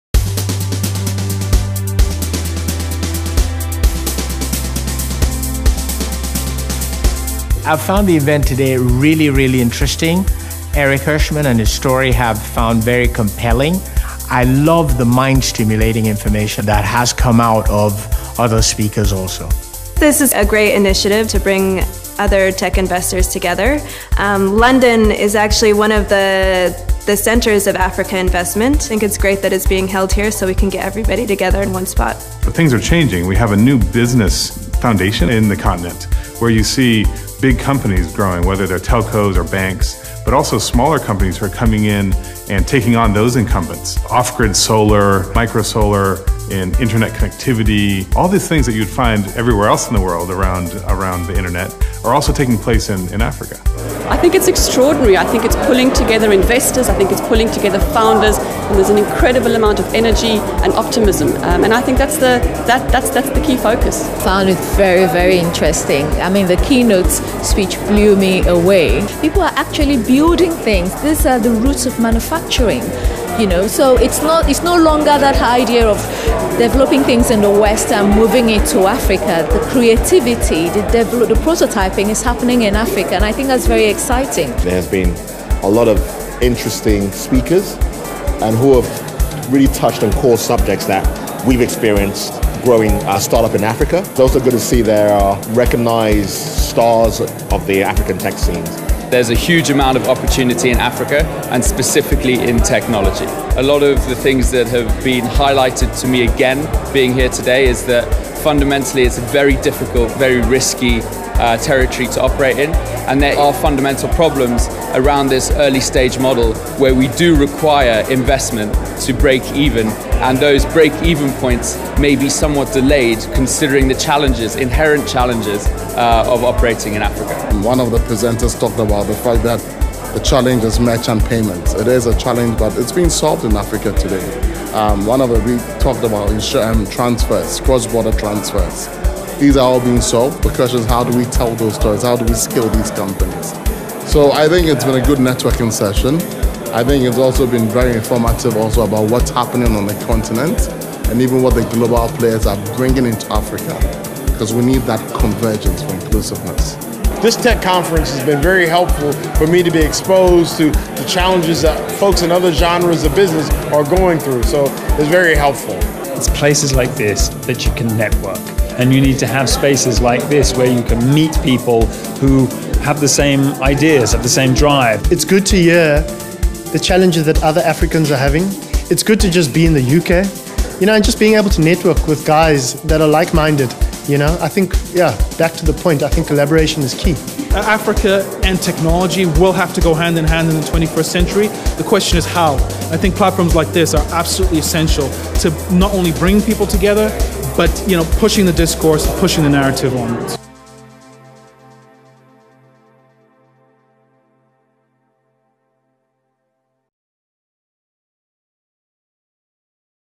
Africa Tech Summit London 2016 Highlights
Technology leaders from across Africa and Europe, international investors, entrepreneurs and leading African start-ups convened at the Africa Tech Summit London 2016 for an exclusive, invitation only conference to discuss the African technology landscape in Sub-Saharan Africa.